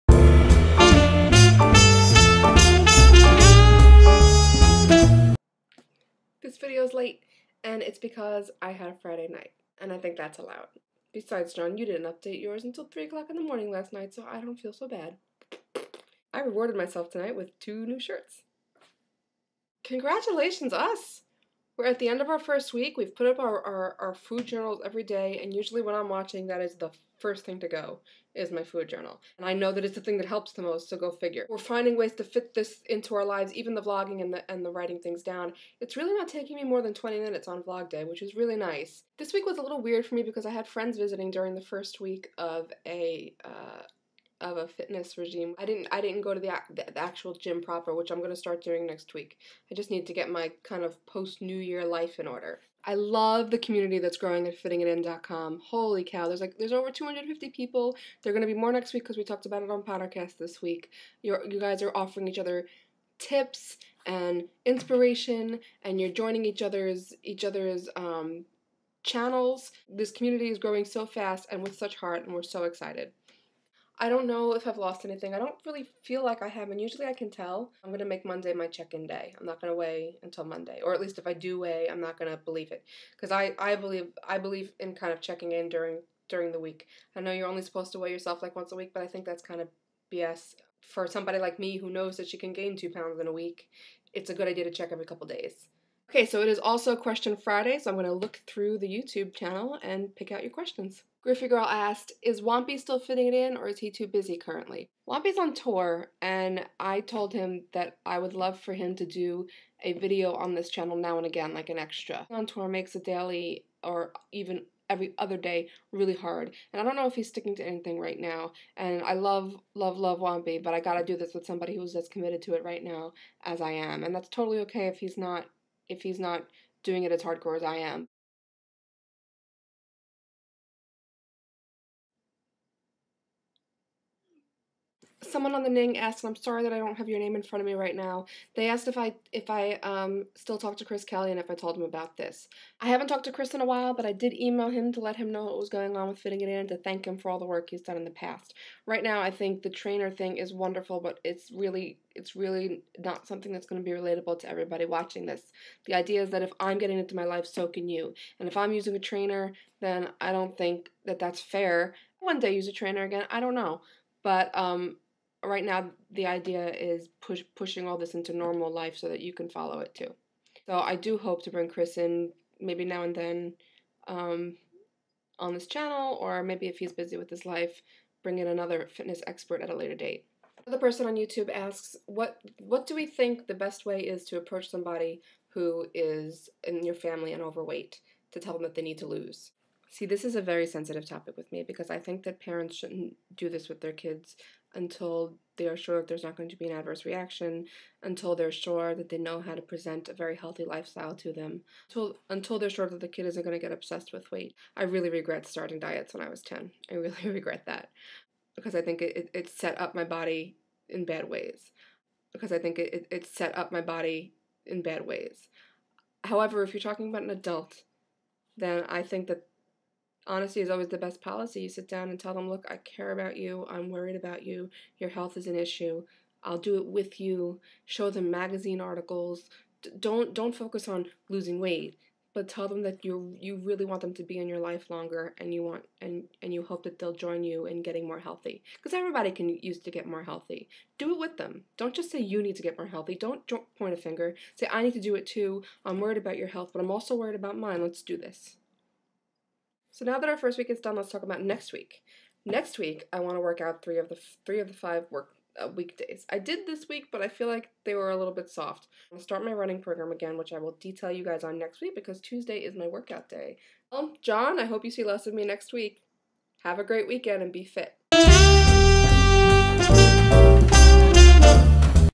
1/9: End of Week 1, woo hoo!
Question Friday, plus plans for next week!

THERE'S A DOUBLE CLIP IN HERE AND I JUST DON'T CARE. SO THERE. It's just a bug. I hope you guys don't mind. :) Rexporting is a pain.